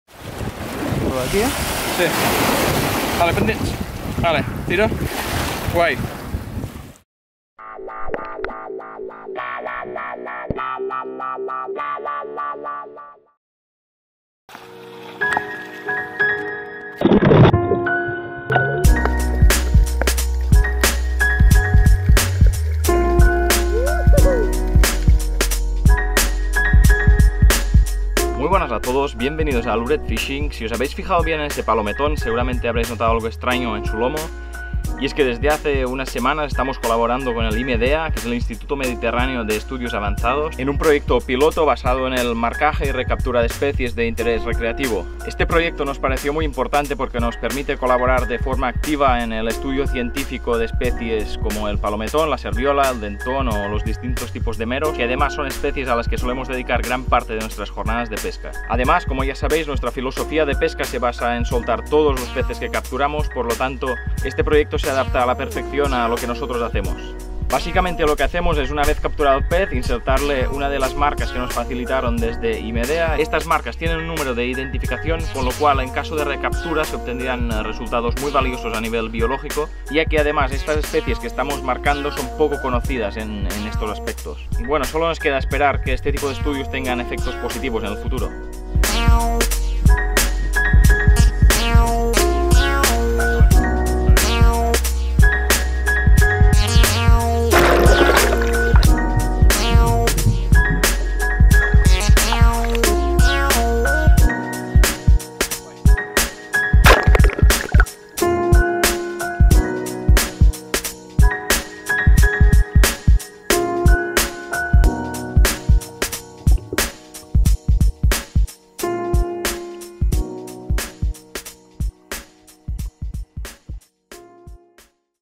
Pescadores ¿científicos? | Captura MARCAJE y suelta
En las últimas semanas estamos colaborando con el Instituto Mediterráneo de Estudios Avanzados (IMEDEA) en un proyecto piloto sobre el marcaje de peces de explotación recreativa. Nos parece muy interesante e importante poder colaborar en el estudio científico a nivel biológico de peces como el palometón, serviola, dentón y meros.

Desde aquí dar las gracias a IMEDEA (UIB - CSIC) y a Pep Alos por la oportunidad de participar y ayudar en la medida de lo posible.

Producido por Lured Fishing.